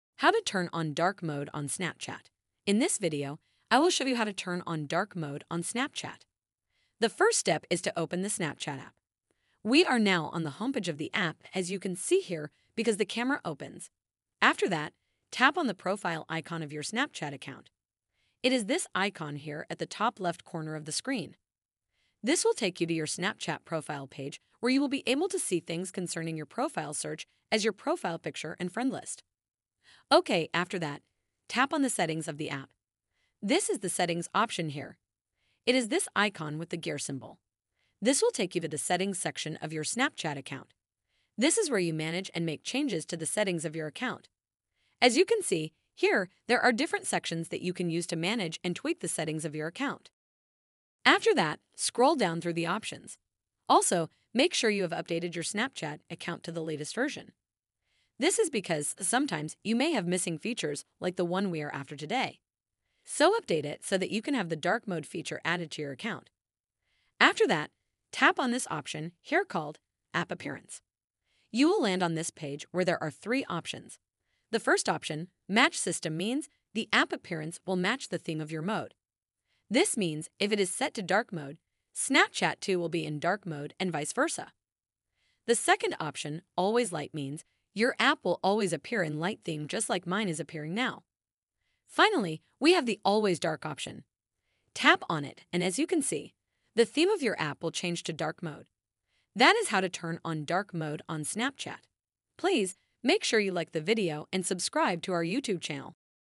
How To Turn On Dark Mode On Snapchat
In this video, I will show you How To Turn On Dark Mode On Snapchat. you will be to learn how to enable dark mode on Snapchat.